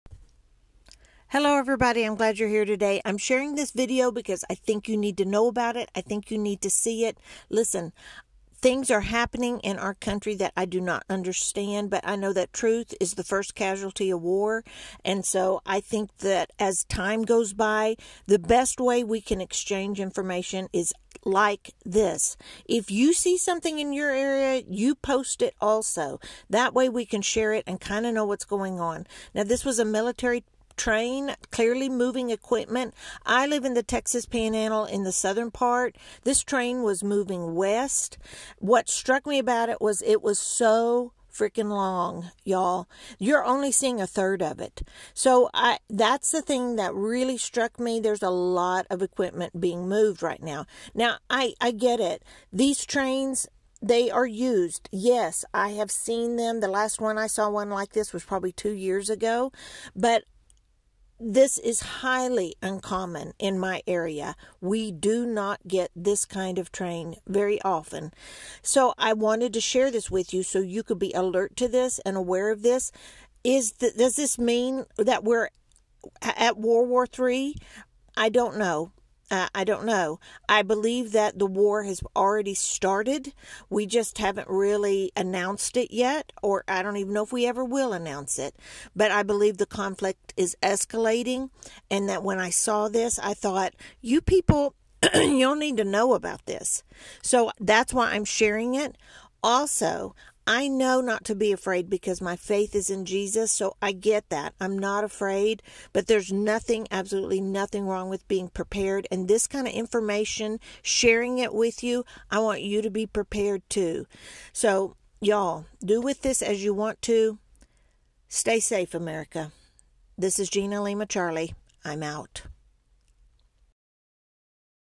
⚠️-WW3 BE AWARE of THIS ⚠️WARNING-- MILITARY MOVEMENT HAPPENING😳-- HUGE WAR VEHICLES IN MY TOWN!
When I saw this I knew I had to get it out there for everyone to see for themselves what's being moved across our country. It may mean nothing...it may mean something...I honestly don't know for sure but I feel it's my duty to get it out to everyone! Stay close to Jesus!